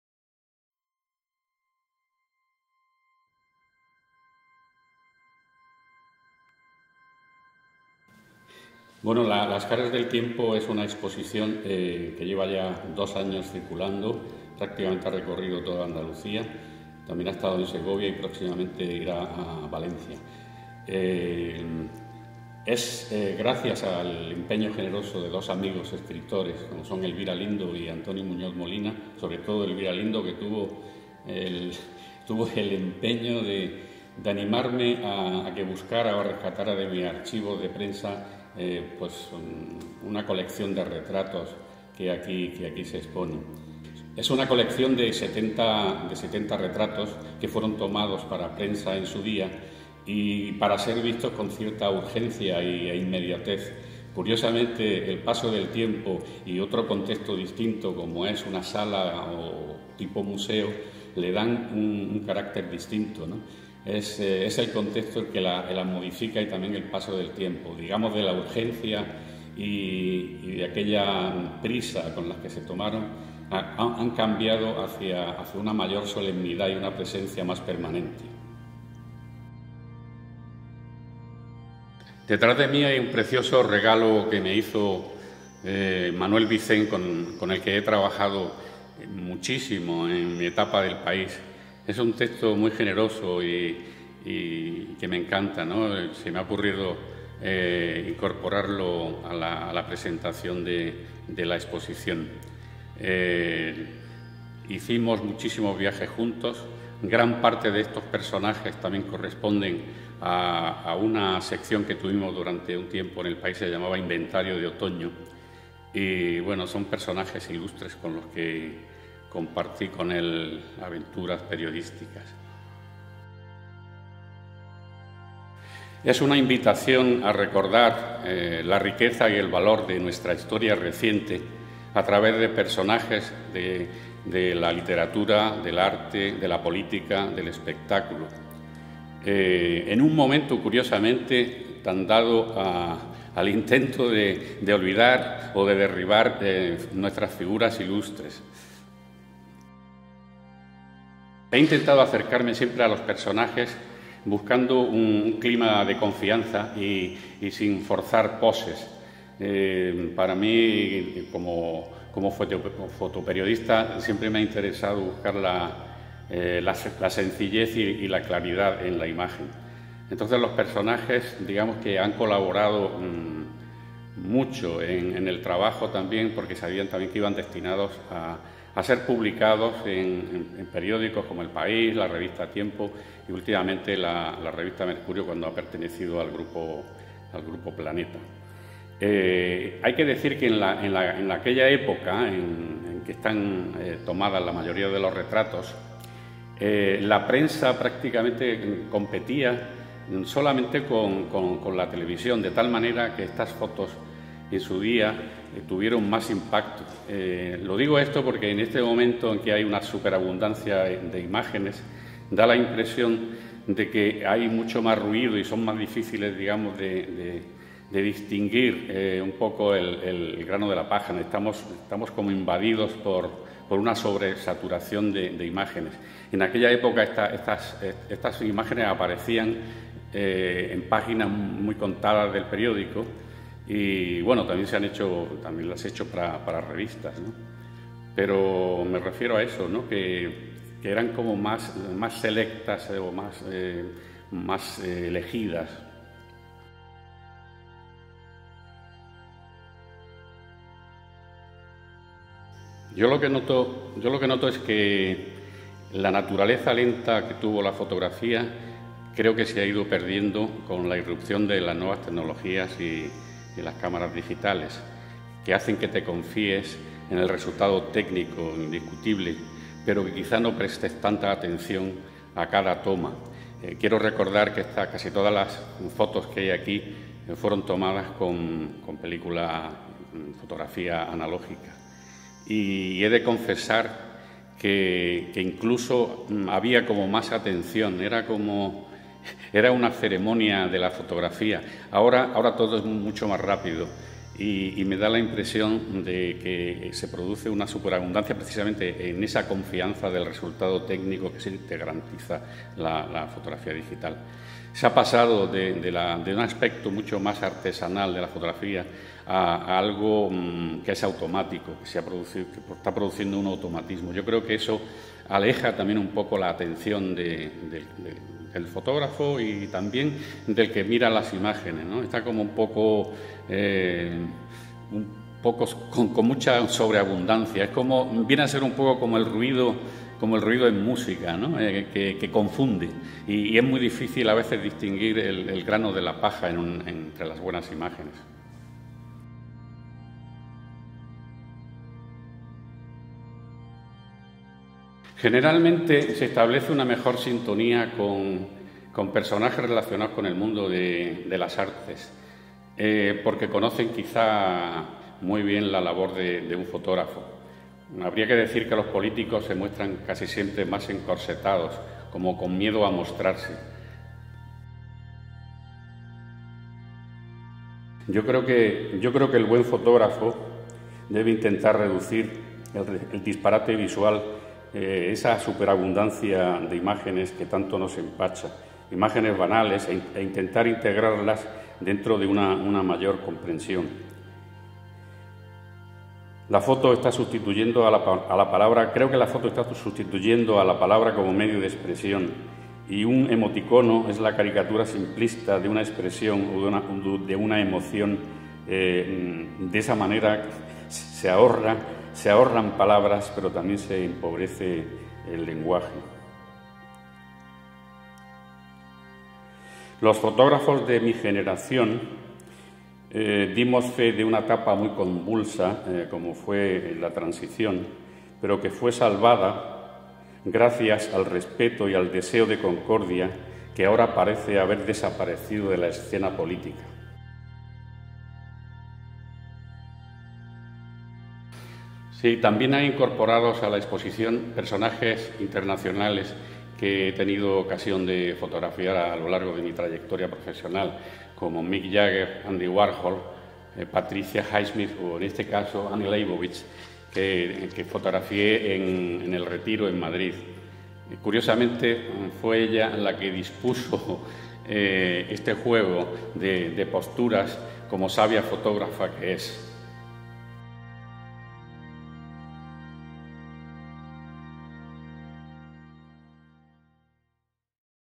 Las caras del tiempo, retratos de Ricardo Martín
Puedes ver la exposición hasta el 19 de septiembre, de lunes a sábado de 11 a 21 horas y domingos y festivos de 11 a 15 horas en la sala Santa Inés de Sevilla.